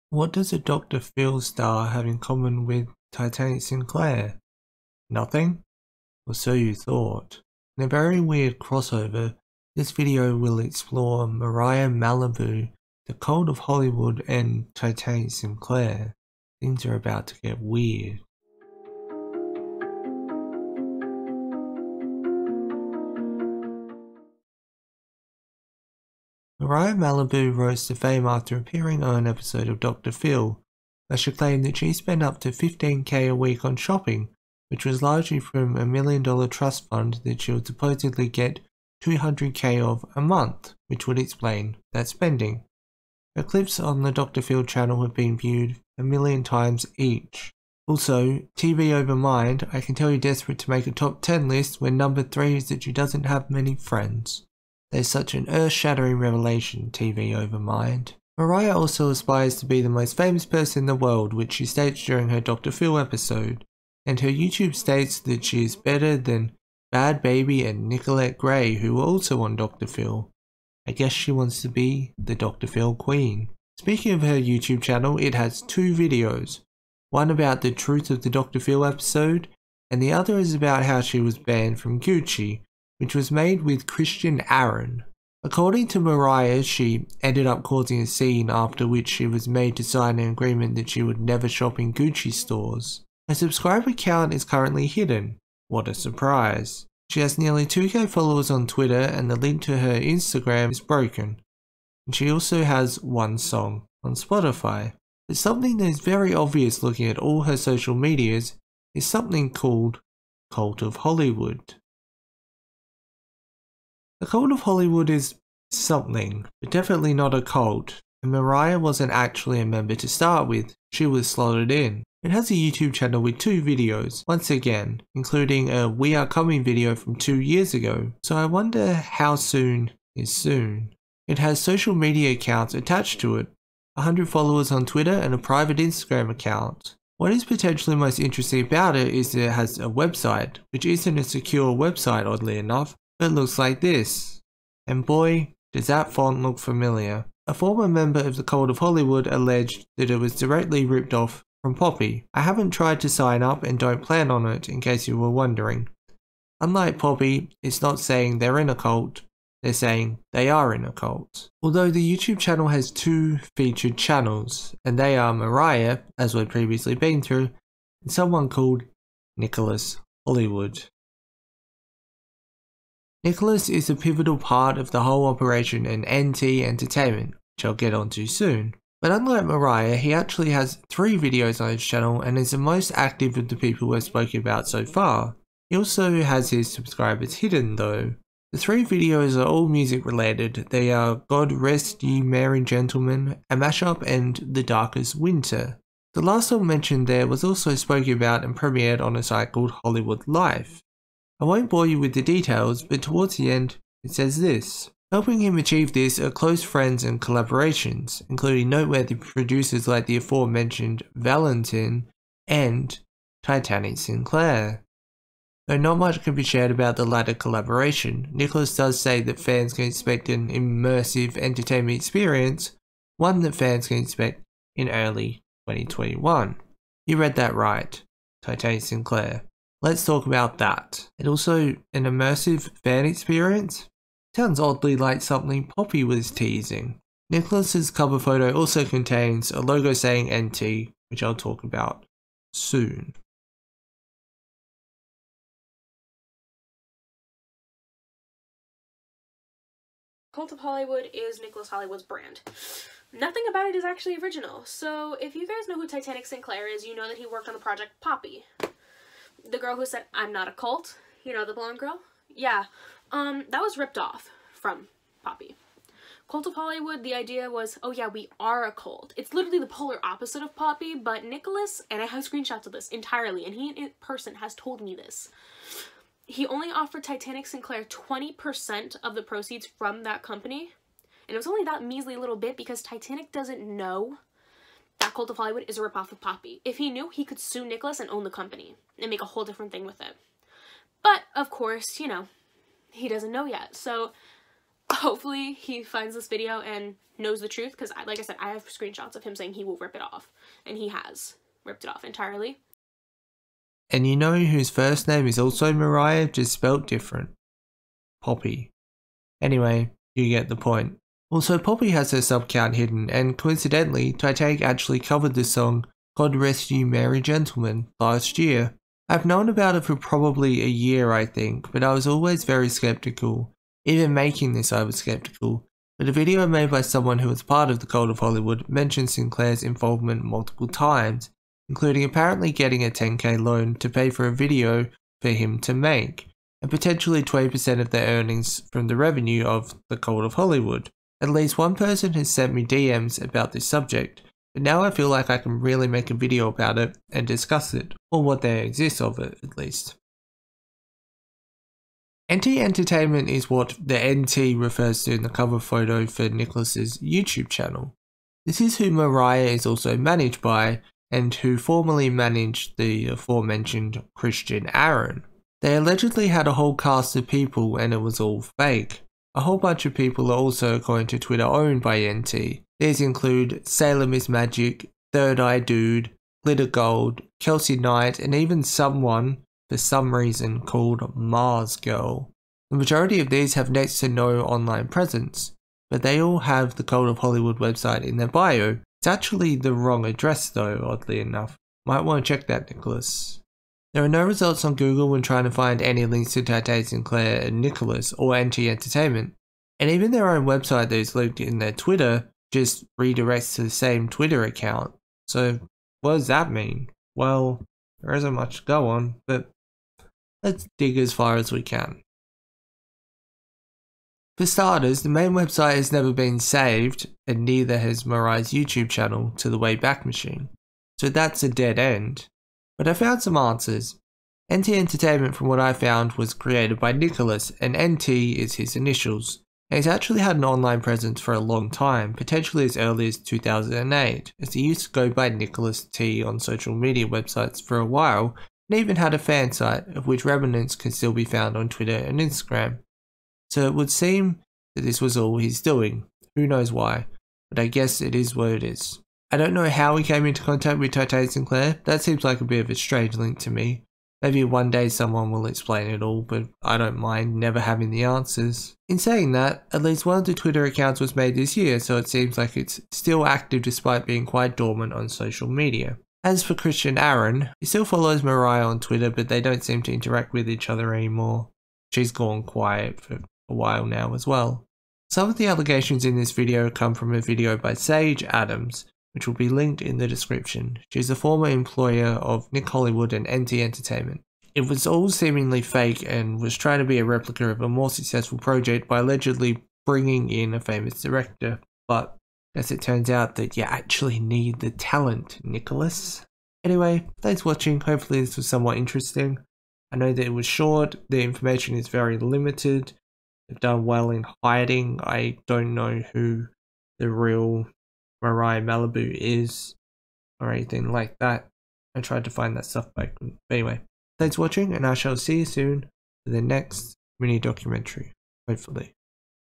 Mariah Malibu: Titanic Sinclair's Secret Project
Titanic Sinclair has had multiple successful projects, including Poppy, Mars Argo and Blame Candy, but what about Mariah Malibu? Mariah rose to fame from Dr Phil and the inclusion of Titanic Sinclair and Nick Hollywood make the whole story really bizarre.